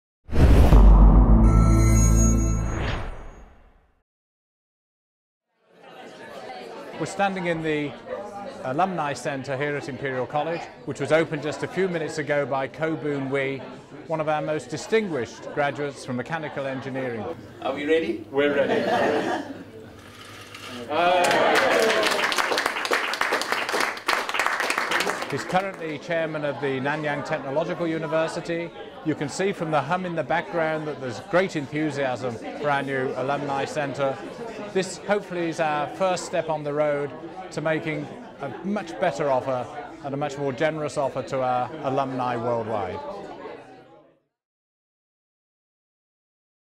Opening of the Alumni Visitor Centre
The official opening of the Alumni Visitor Centre at Imperial College London by Mr Koh Boon Hwee (Mechanical Engineering 1972)